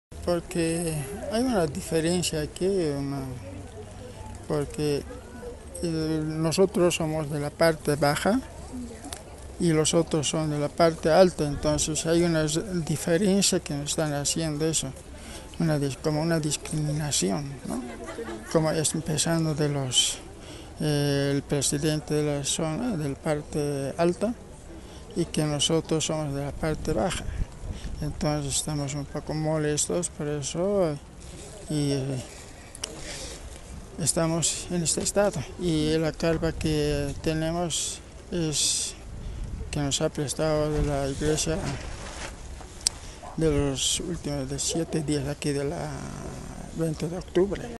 Historia de un Derrumbe II
Otro vecino, sin embargo, relata que hay una diferencia entre los vecinos, que los pertenecientes a la parte alta del deslizamiento reciben mejores tratos en el campamento de la Cancha Fígaro, sin embargo, ellos que vivían de la parte alta les tocó habitar en este sector, en una carpa pequeña que fue donada por una iglesia.